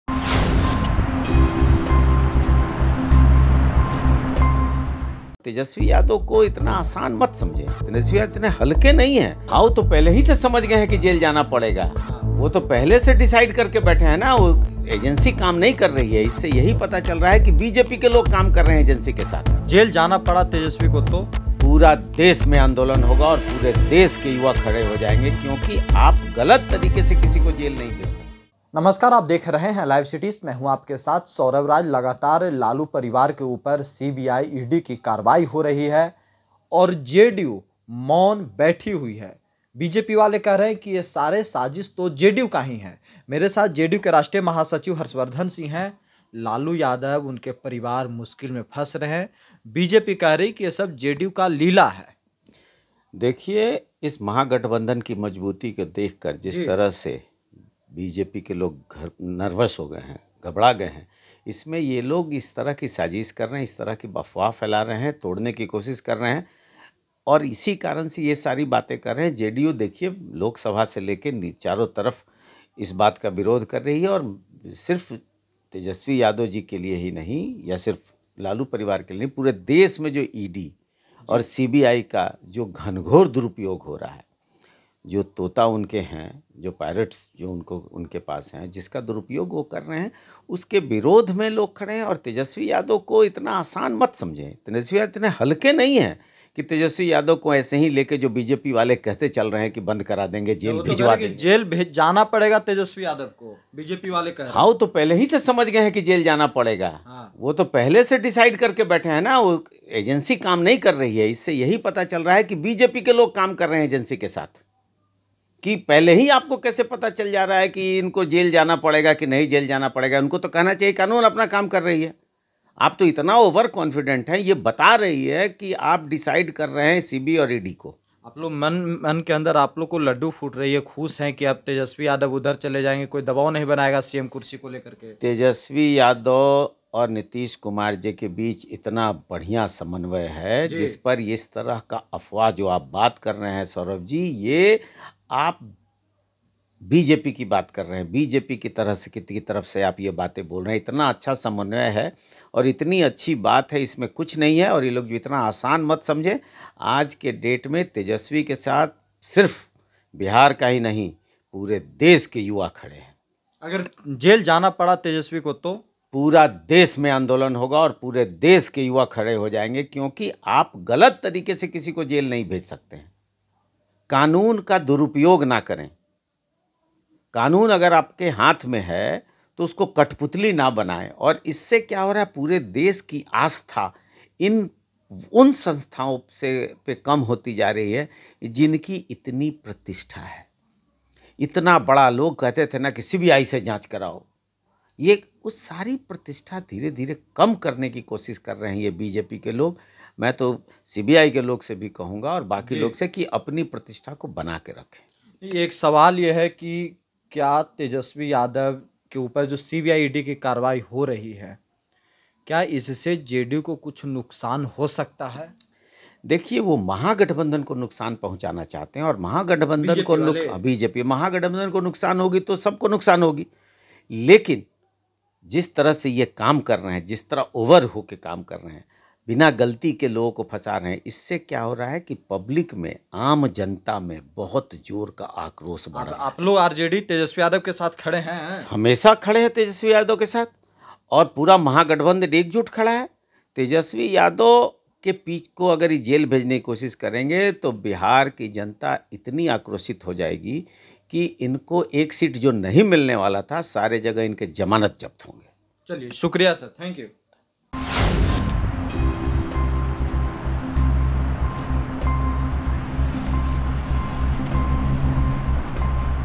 Tejashwi Yadav को कुछ भी हुआ तो परिणाम बुरा होगा, JDU ने कह दिया, BJP सोच ले, सुनिए | Bihar News
Tejashwi Yadav को कुछ भी हुआ तो परिणाम बुरा होगा, JDU ने कह दिया, BJP सोच ले, सुनिए | Bihar News | LiveCities